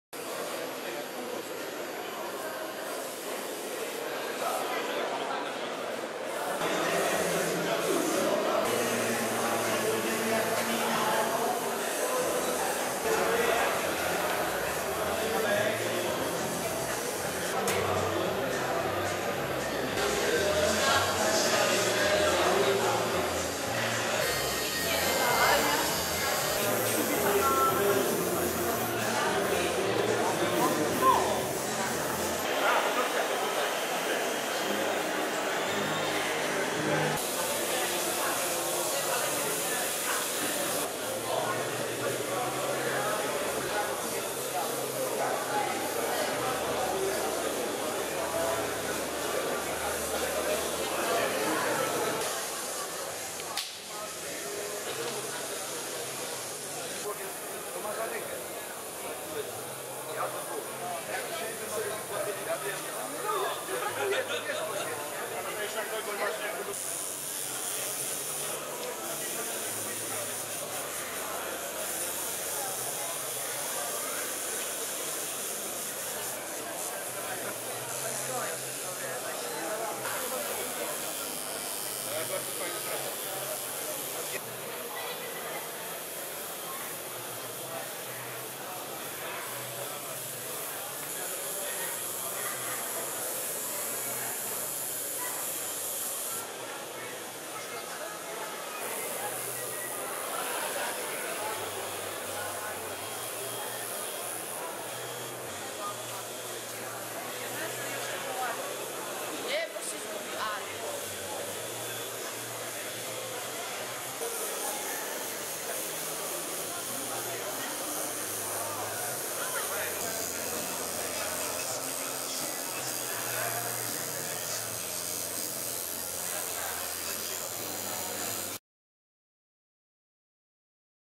Tatoo Warsaw - Festiwal Tatuaży w Warszawie
Jak upadek komunizmu wpłynął na "dziary"? Tego można się było dowiedzieć na Festiwalu Tatuażu, który 5 i 6 września odbył się w dawnej Wytwórni Wódek Koneser.